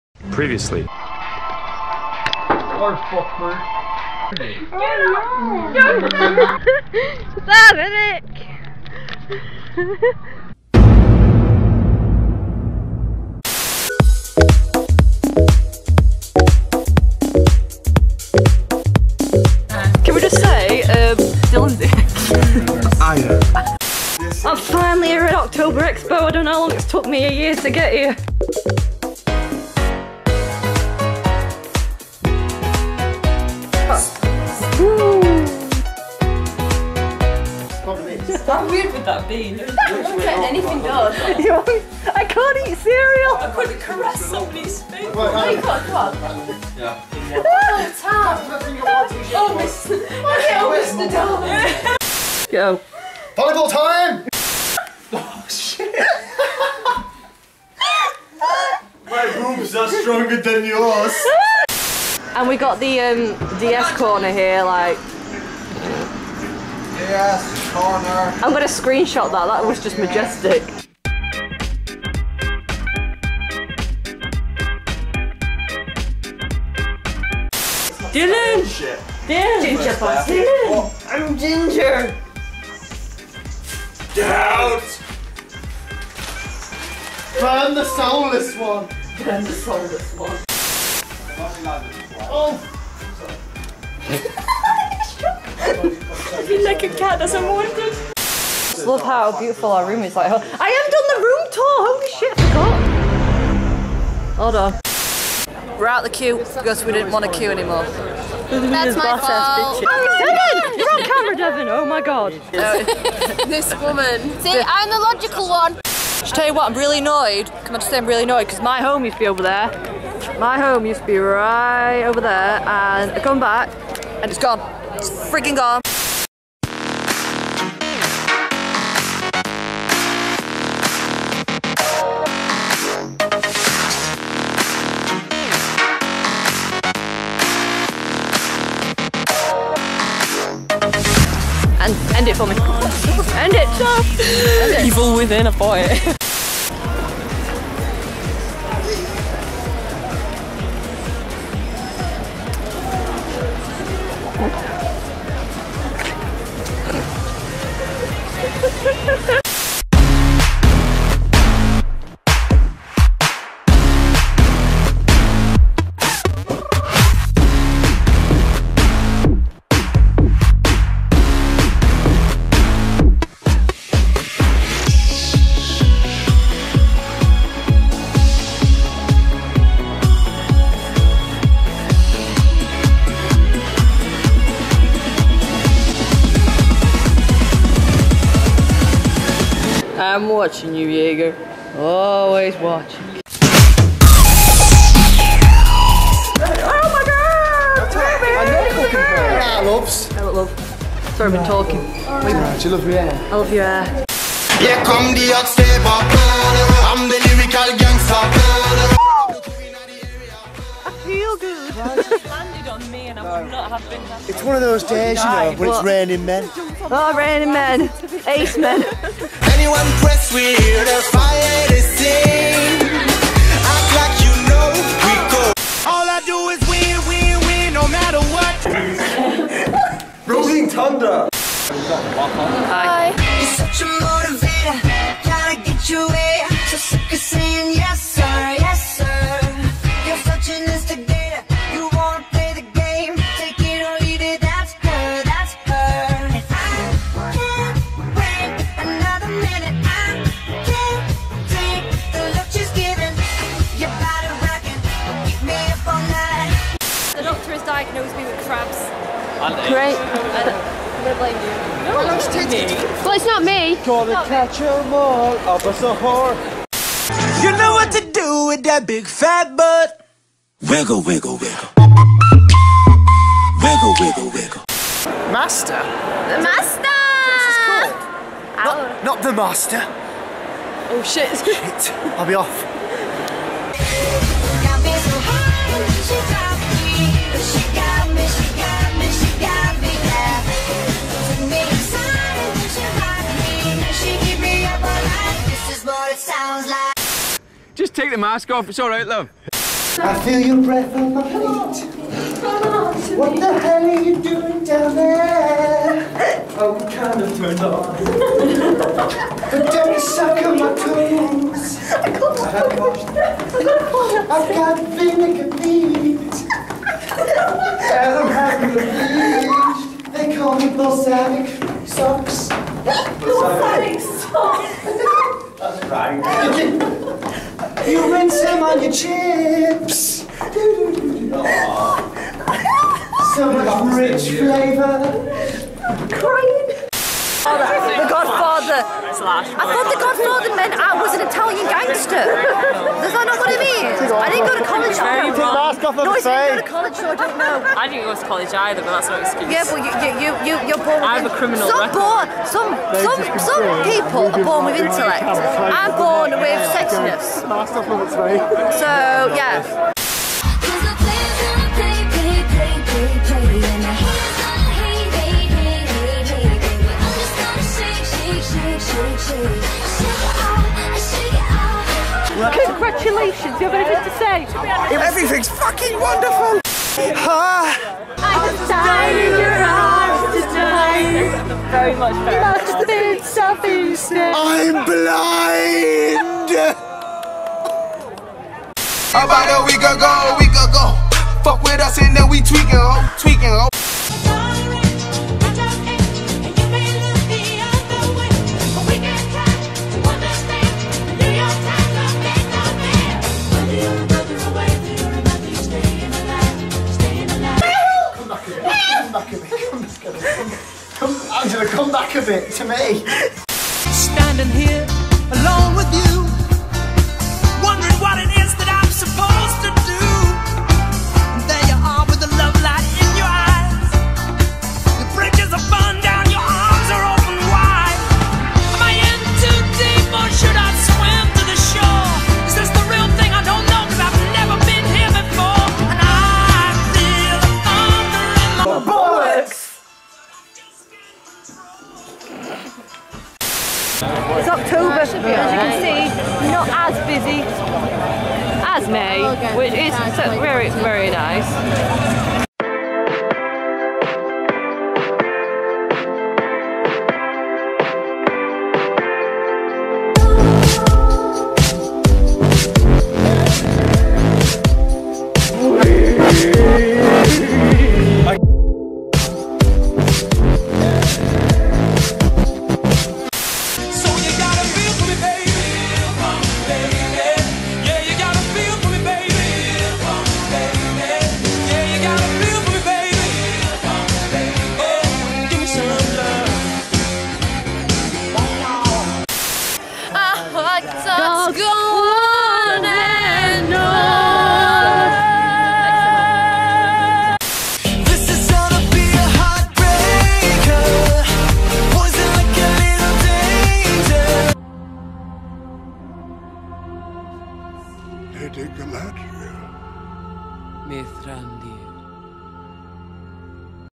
LONDON COMIC CON || MCM EXPO OCTOBER 2014
One last expo to end the year on ;)
We had tons of fun at October Expo and this video is just a lot of crack and what even xD

Emma xxx